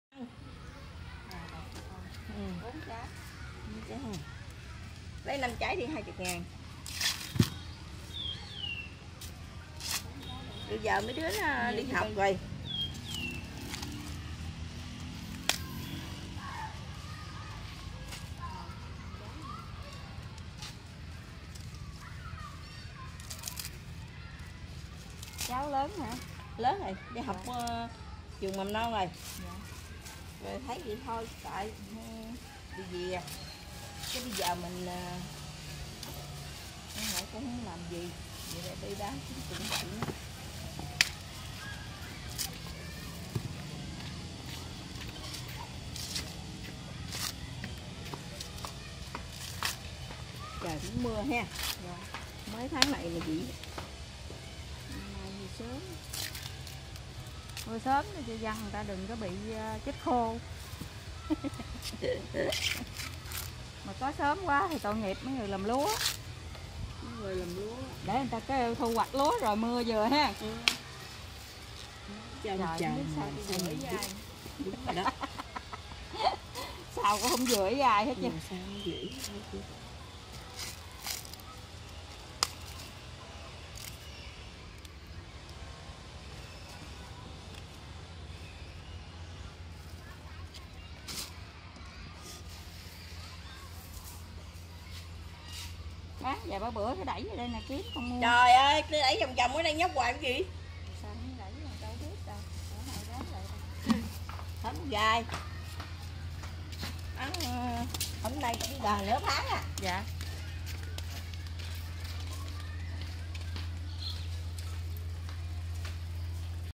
Duyên Mua Bắp Tươi
Quay trước nhà trên dường Trường Tiêu Học B Bình Hoà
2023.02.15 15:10
Phạm vi công cộng